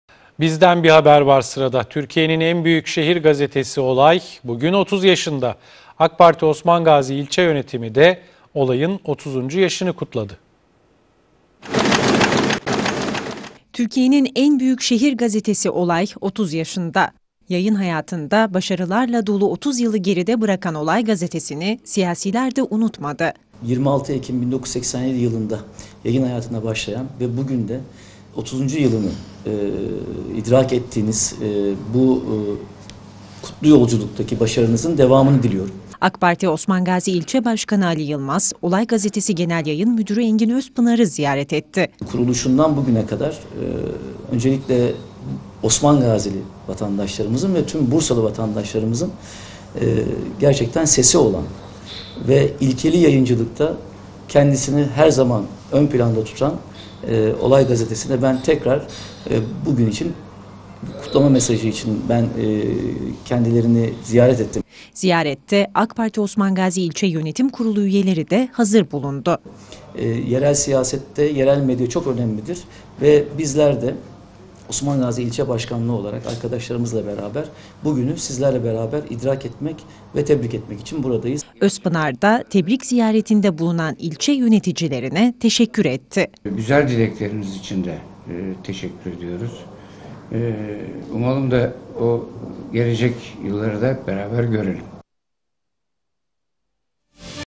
Olay Gazetesi 30 yaşında (26 10 2016)
Türkiye'nin en büyük şehir gazetesi Olay 30 yaşında. AK Parti Osmangazi ilçe yönetimi de Olay'ın 30. yaşını kutladı.